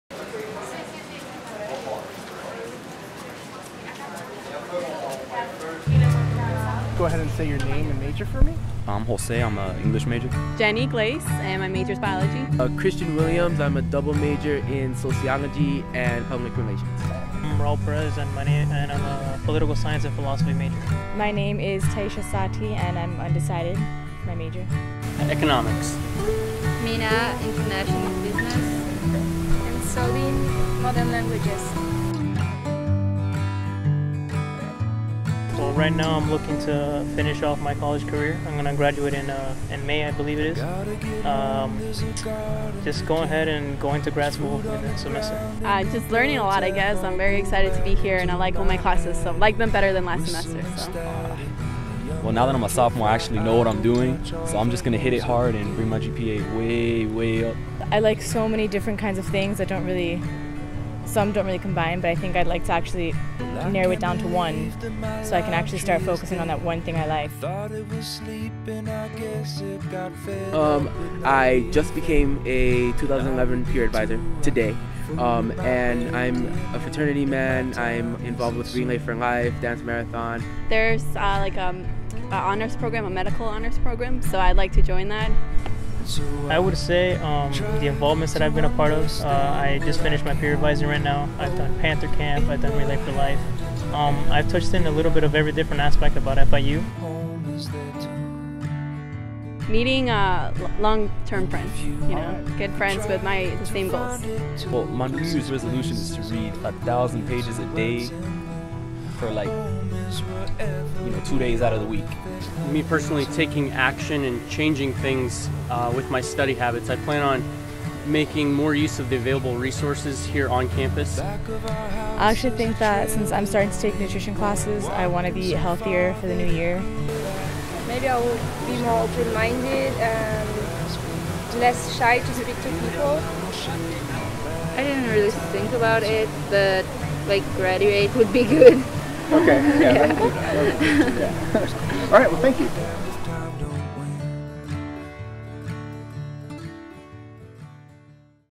Campus Talk - Spring 2011
To kick off the Spring semester, the news.FIU.edu team wanted to get to know FIU students better; so videograpghers Doug Garland and Chris Necuze fanned out around MMC to find out what was on their minds. Meet some of your fellow Panthers in Campus Talk, an FIU web-series.

Submit it to the site. If we like it, we'll post it.